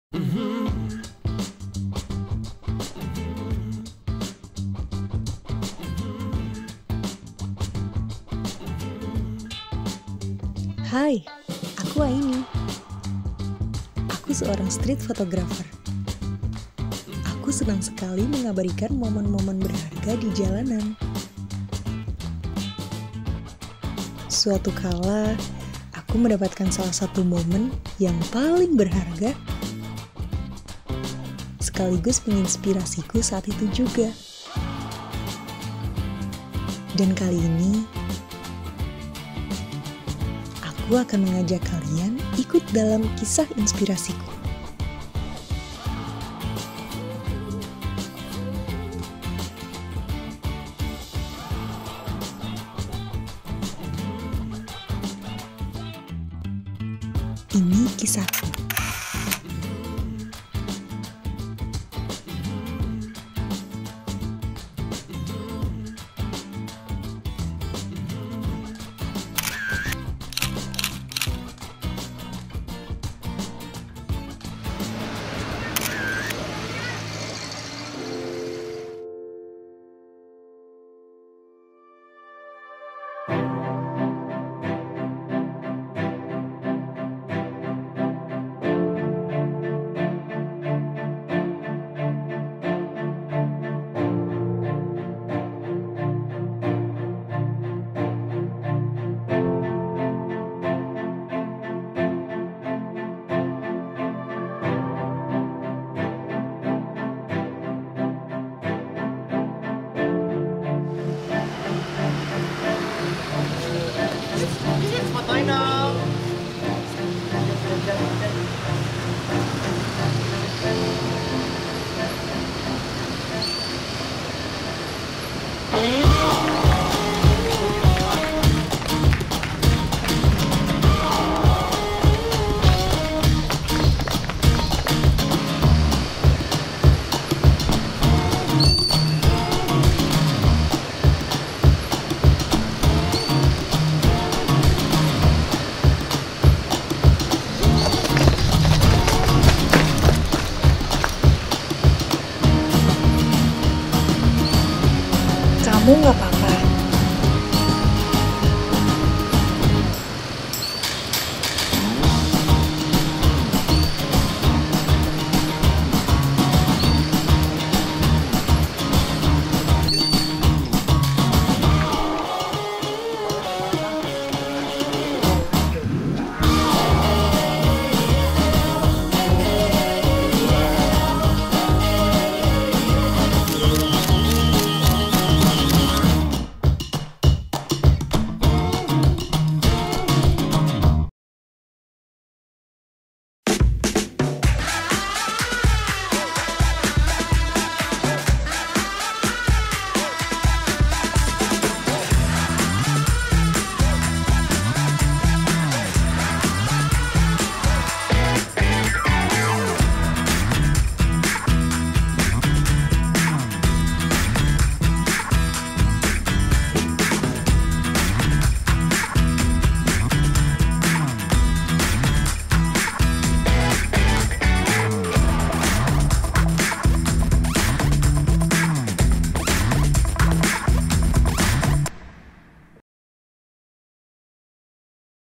SIAPAKAH KAMU? ~ Sebuah Kisah Inspiratif ~ Inspiring Goodness Eps. 1
Aini adalah seorang Street Fotografer yang sangat lincah untuk mengabadikan moment. Suatu saat ia mengalami kejadian yang sulit dan menegangkan. Mau tau kisah lengkapnya? Yuk simak videonya